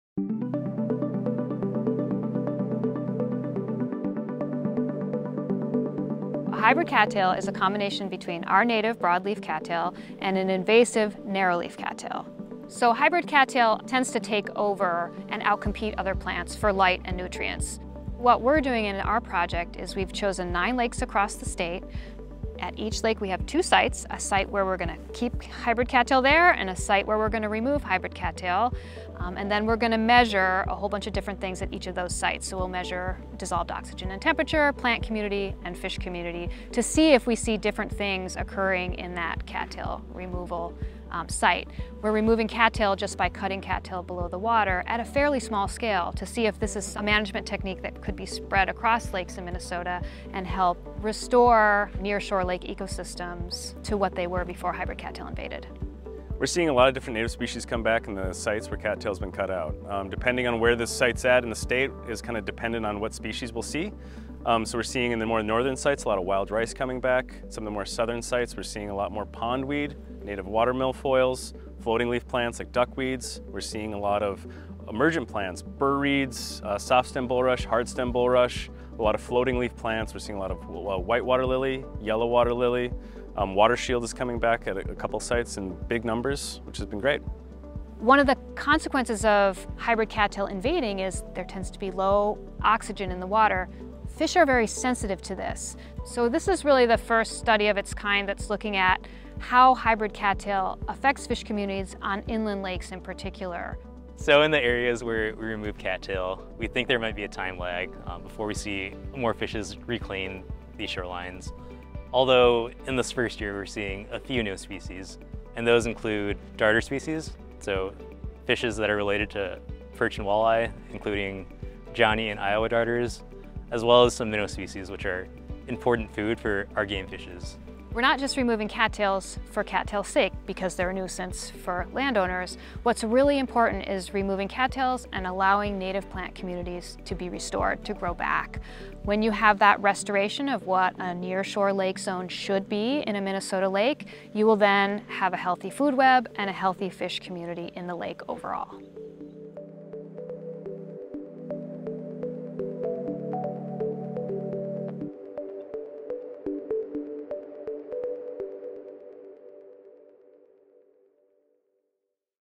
Enhancing Habitat and Diversity in Cattail-Dominated Shorelines
This project aims to quantify and clarify whether or not hybrid/narrowleaf cattail removal can increase plant diversity and benefit fish communities in nearshore lake ecosystems and how these effects vary regionally in Minnesota.